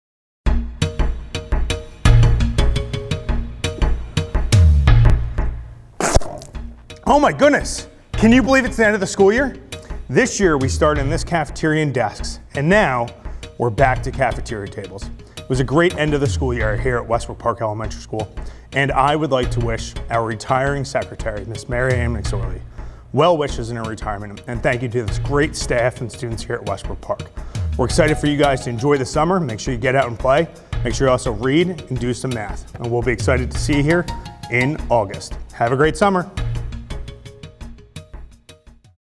Westbrook Park Elementary School End of the Year Message 2022
An end of a year message from Principal Comfort!